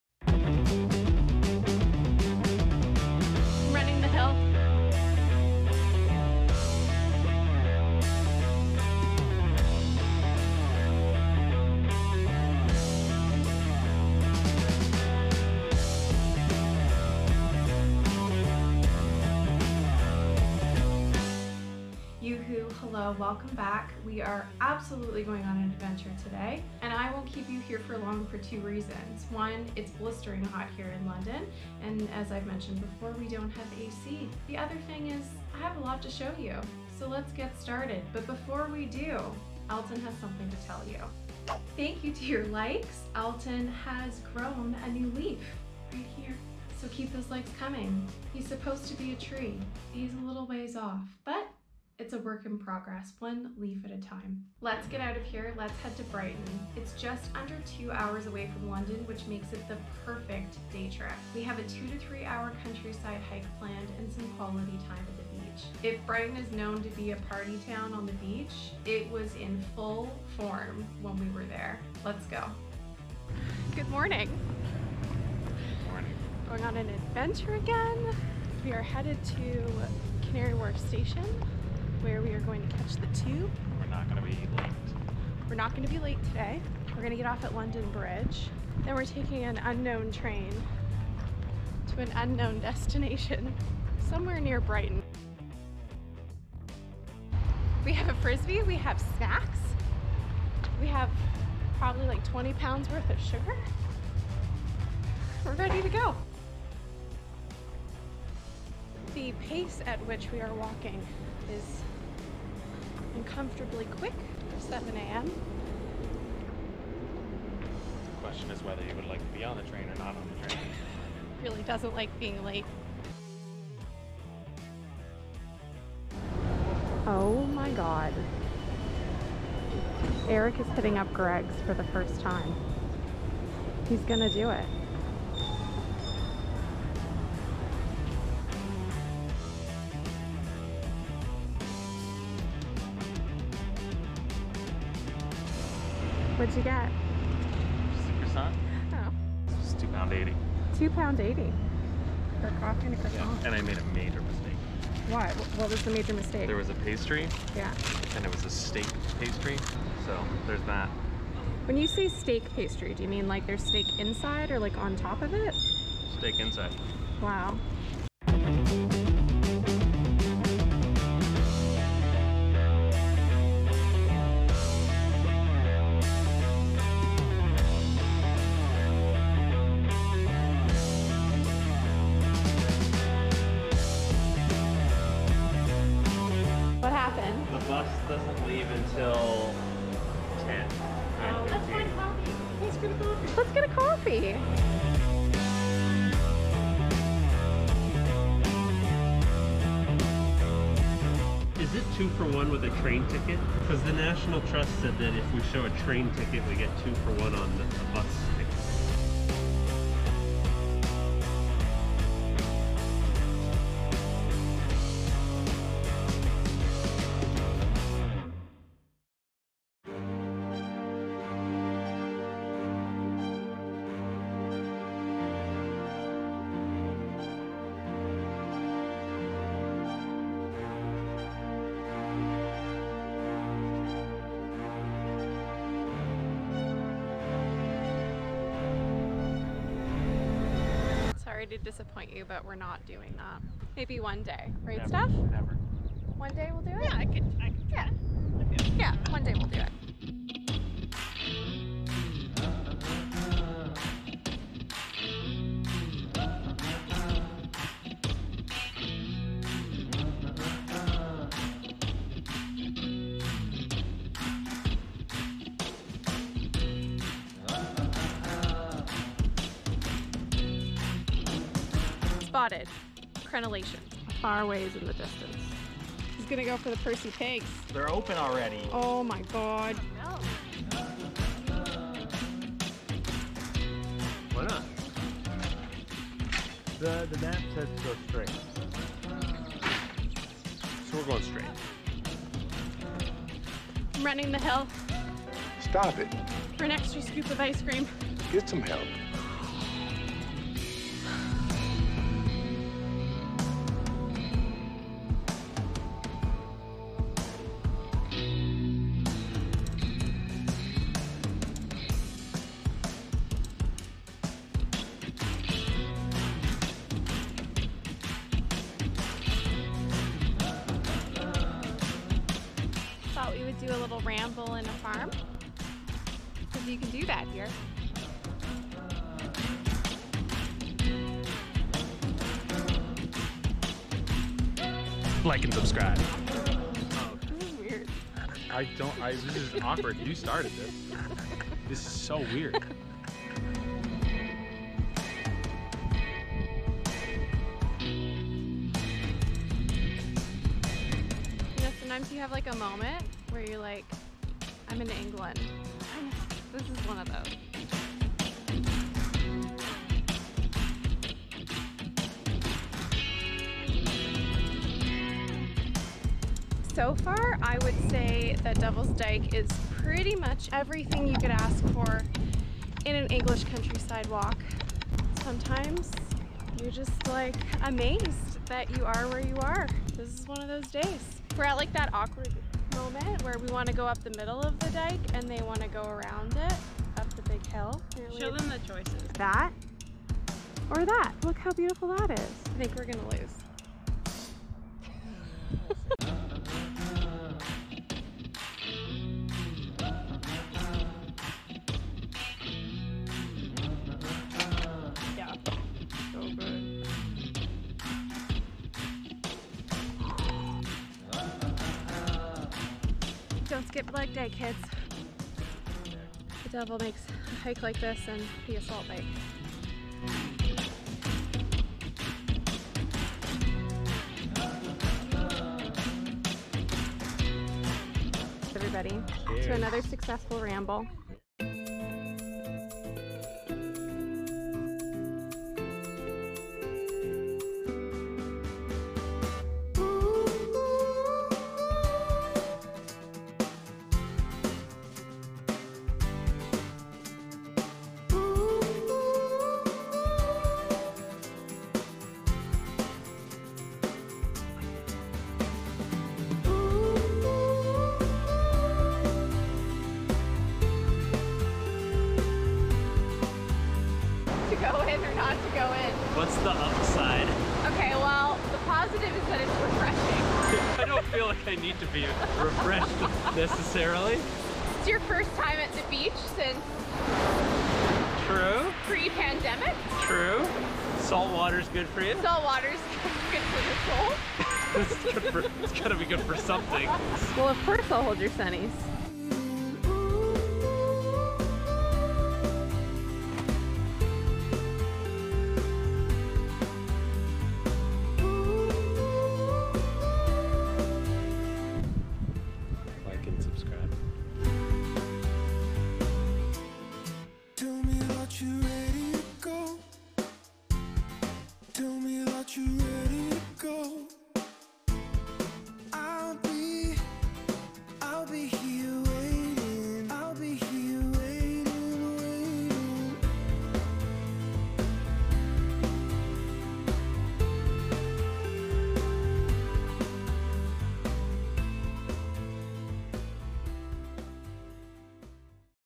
DEVIL'S DYKE HIKE & BRIGHTON BEACH
The PERFECT day trip from London. Devil’s Dyke and Brighton Beach are a must-do… and under a 2 hour trek from London (by train)! Join us as we take you through the rolling country-side and chill out on the beach before heading back home.

This adventure has been our favourite so far!

0:00 - Devil's Dyke and Brighton Beach Day Trip
2:03 - Eric get's Gregs for the first time
3:10 - Great coffee spot in Brighton (Blend & Brew)
3:40 - Devil's Dyke
5:17 - Cows!
7:12 - Brighton Beach

If you enjoyed this content, please give that like button some love.

Michelle Christine

(EP. 7)